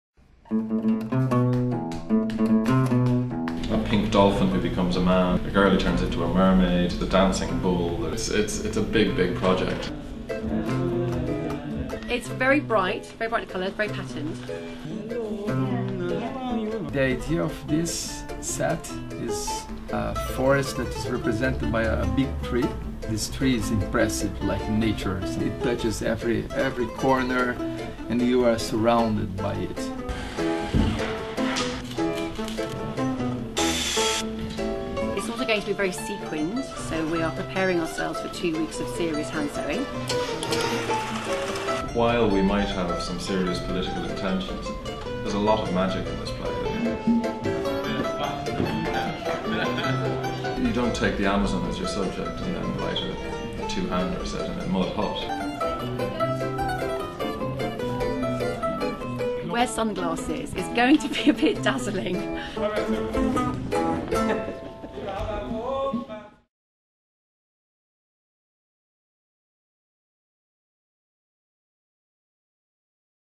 This Christmas - Amazonia!
Music and dance... laughter and adventure.
Christmas, Brazilian style!
'It doesn't get more spectacular, sensory or right-on than the
Young Vic's bastion of first-rate Christmas fare' Daily Telegraph